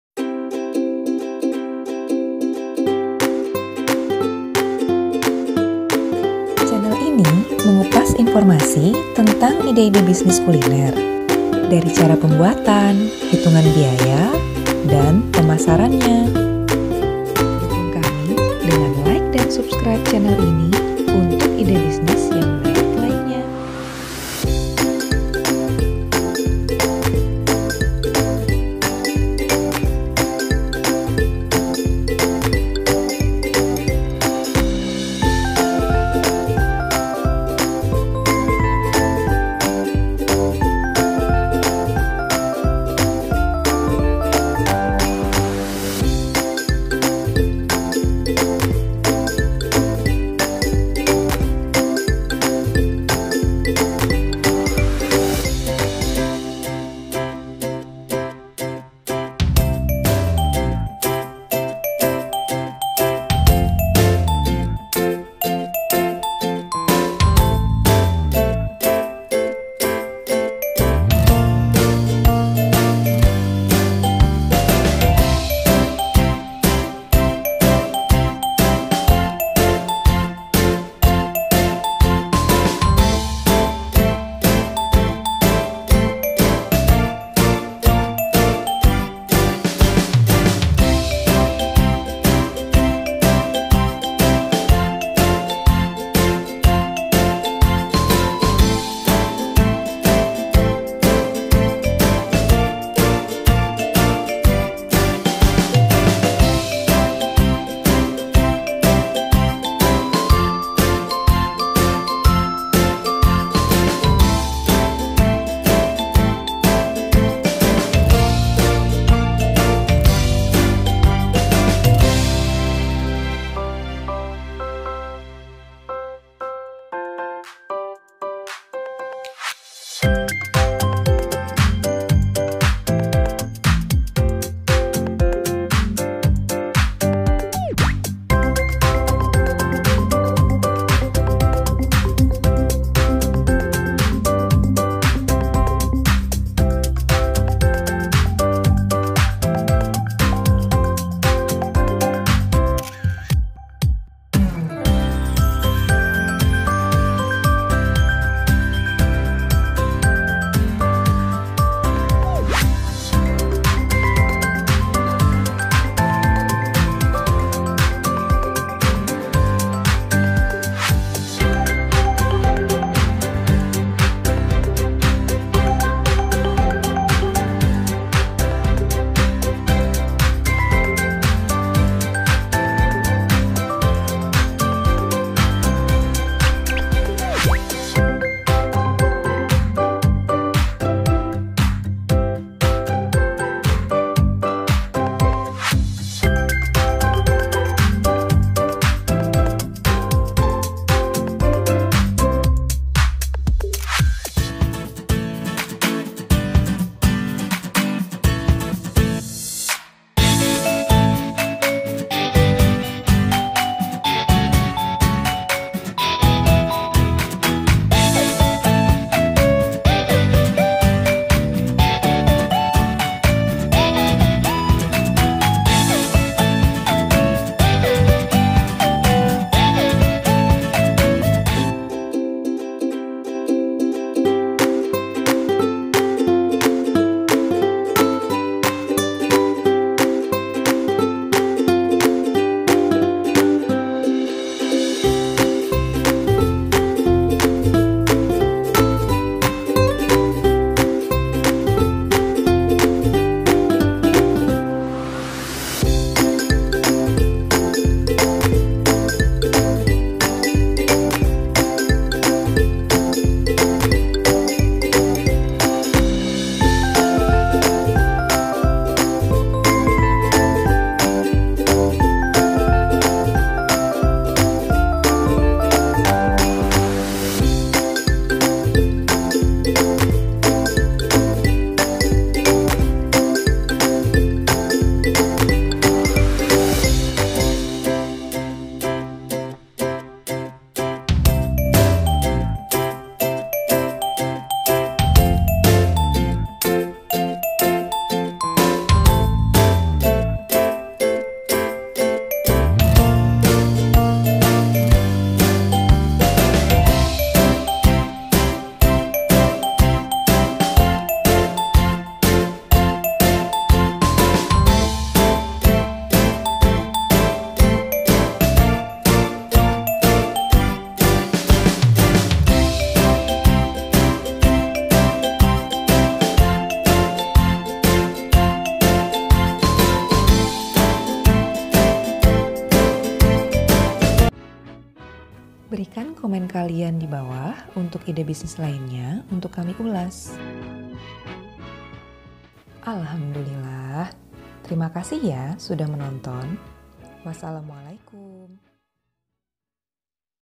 CHOCO BALL COOKIES | IDE BISNIS KULINER
Tak terasa lebaran tinggal menghitung hari saja. Lebaran identik dengan aneka suguhan makanan yang variatif, salah satunya adalah  kue kering/cookies.

Kali ini kami mengulas Choco Ball Cookies, salah satu varian kue kering yang sangat diminati anak-anak.

Selamat mencoba ☺

* Mohon maaf pengambilan gambar ala kadarnya 😅